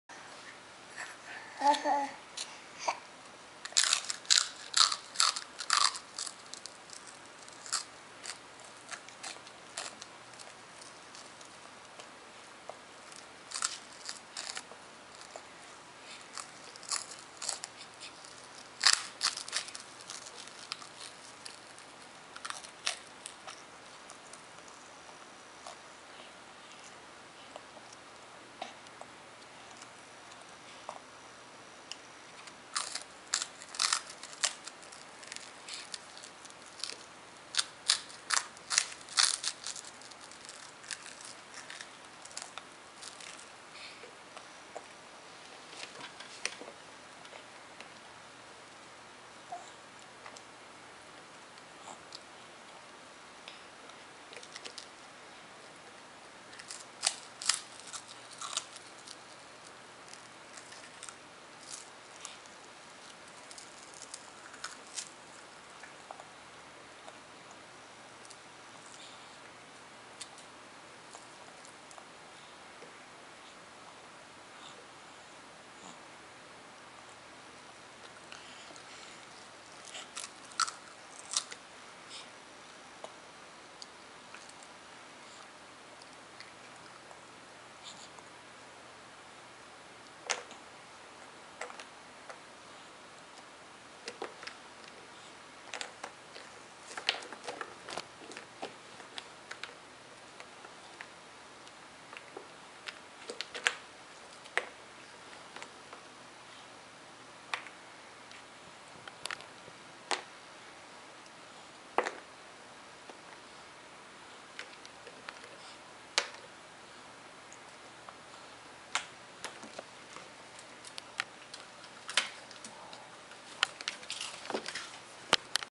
Conor Loves Baby Mum Mums
Conor is methodical when eating Baby Mum Mums.  First he eats one side, then the other.  Of course some ends up on the floor for Lucky and Lola.